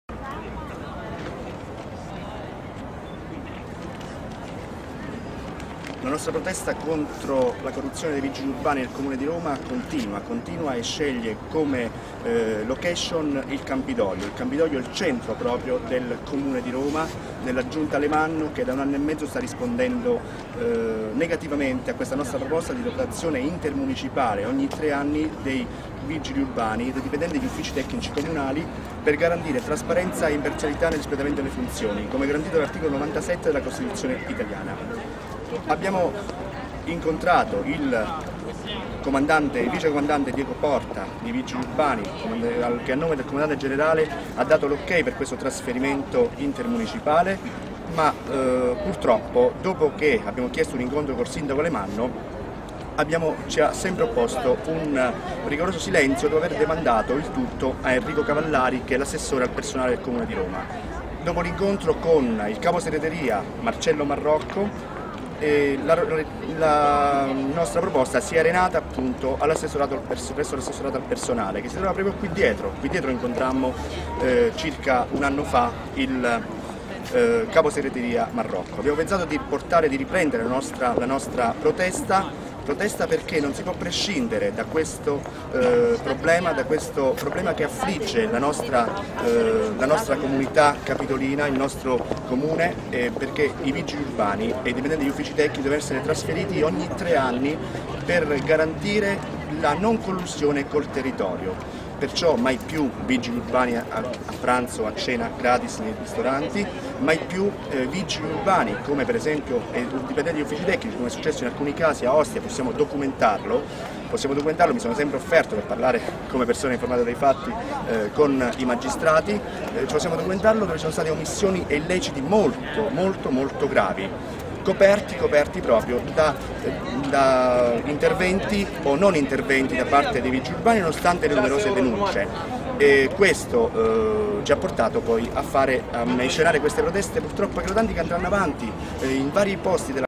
Antonello De Pierro incatenato al Campidoglio contro la corruzione dei vigili urbani a Roma
Il presidente dellItalia dei Diritti: Puntiamo a salvaguardare luguaglianza dei cittadini davanti alla legge e a tutelare limmagine stessa del corpo dei vigili urbani

Roma Proseguono le manifestazioni di protesta dellItalia dei Diritti e del suo presidente Antonello De Pierro, per richiamare lattenzione di istituzioni e opinione pubblica sulla delicata questione della rotazione intermunicipale dei vigili urbani e dei dipendenti degli uffici tecnici comunali di Roma. Il leader del movimento nazionale a tutela dei cittadini, dopo gli incatenamenti della scorsa settimana davanti alle sedi capitoline de Il Messaggero e de La Repubblica, sceglie questa volta come luogo simbolo della sua protesta il Campidoglio. Questa manifestazione acquista un particolare valore proprio perché avviene a casa del sindaco Alemanno dichiara polemico De Pierro . Noi non ci fermeremo con la nostra iniziativa, specie alla luce della recente indagine sullUfficio Condono Edilizio. Per noi questa è lunica via possibile per chiedere che sia garantita trasparenza e imparzialità da parte dei vigili urbani e degli impiegati delle U.O.T., spesso esposti a tentazioni di collusioni sul territorio che li portano ad avere comportamenti riconducibili a omissioni e illeciti di vario genere. Non siamo noi a dirlo ma i provvedimenti della magistratura. E a nostro avviso la questione è ancora più grave, anche se le mele marce sono poche rispetto alla maggior parte di coloro che svolgono il proprio lavoro con impegno e diligenza. Poi, il numero uno dellItalia dei Diritti sottolinea gli obiettivi concreti che si prefigge di raggiungere con la sua battaglia: Puntiamo a salvaguardare soprattutto luguaglianza dei cittadini davanti alla legge; inoltre, vogliamo tutelare limmagine stessa del corpo dei vigili urbani e lintero apparato dellamministrazione comunale. Per questo motivo chiediamo il trasferimento dei vigili da un municipio allaltro ogni tre anni. Anche se tale provvedimento non risolverà del tutto il problema chiarisce De Pierro di sicuro ridurrà notevolmente le possibilità di perpetrare tali reati. Purtroppo i casi che vengono alla ribalta sono una piccola quantità rispetto al numero reale degli illeciti commessi, che in gran parte restano impuniti. La serie di incatenamenti di protesta da parte del presidente dellItalia dei Diritti andrà avanti anche nei prossimi giorni toccando alcune zone particolari della città, compresa Ostia e le aree limitrofe, come fa sapere lui stesso: Oltre che nei più rilevanti siti istituzionali sparsi nella Capitale, le nostre proteste si concentreranno molto in alcune sedi emblematiche del XIII municipio dove siamo a conoscenza di situazioni assurde e paradossali, delle quali sono sempre pronto a parlare con eventuali magistrati e inquirenti, che coinvolgono in attività illecite alcuni dipendenti di questi comparti professionali. Infatti, in base al numero delle nostre segnalazioni, abbiamo motivo di credere che questo territorio sia più esposto a illiceità di questo tipo. A tal proposito De Pierro annuncia incatenamenti presso la sede della Giunta del XIII Municipio, presso il comando locale della Polizia Municipale e dellufficio tecnico territoriale.